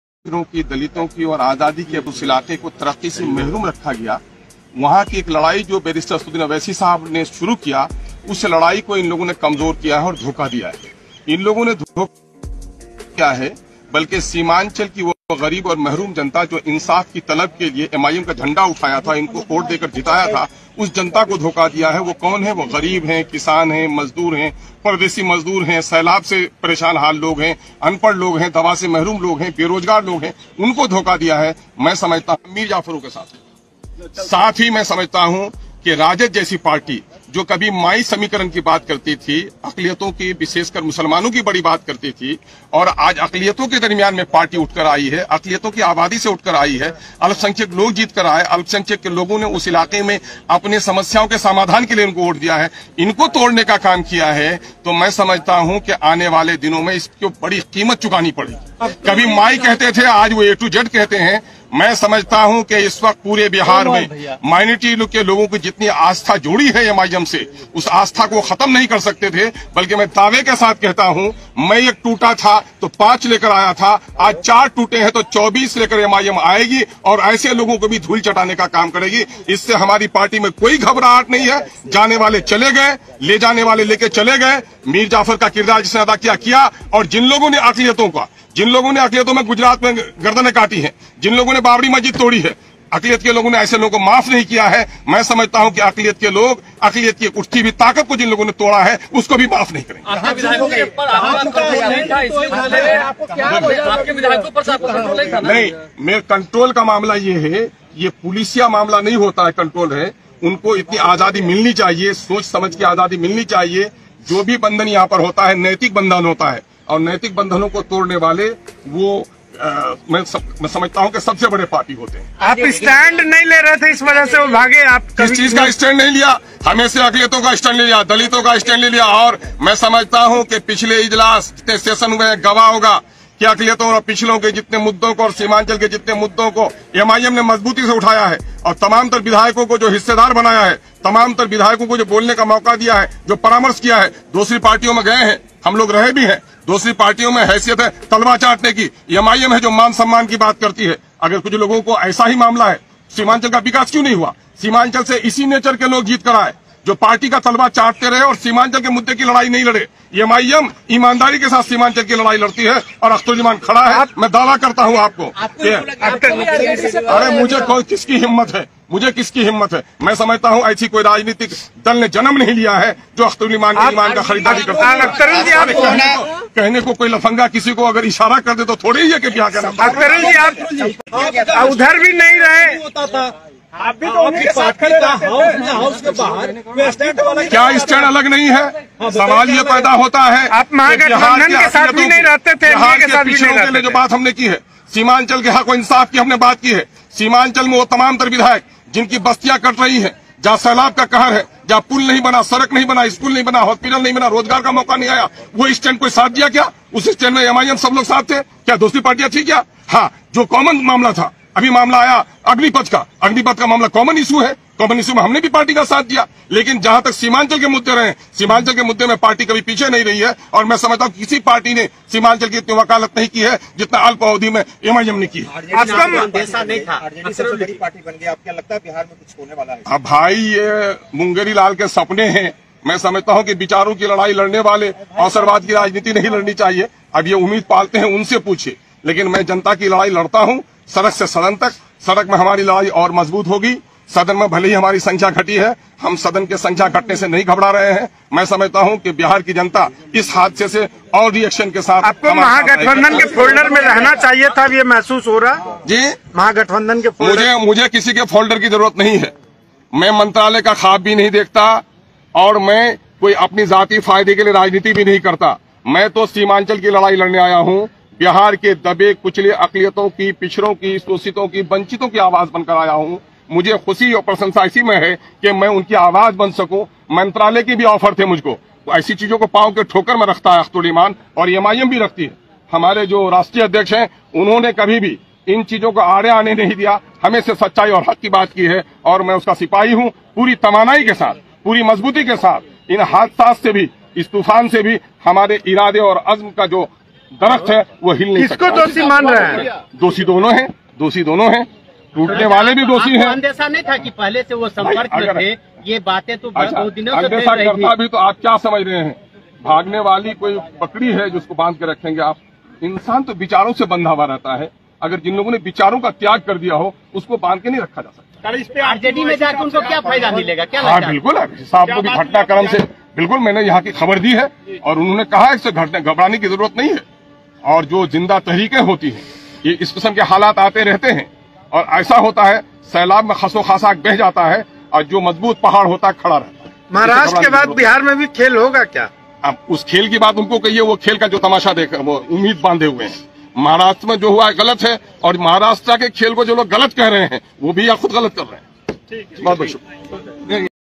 AIMIM के विधायक RJD me जाने पे अख्तरुल ईमान क्या कहा? आए जाते हैं
AIMIM UP&BIHAR🔔👈
AIMIM NEWS
aimim news today
aimim join Rjd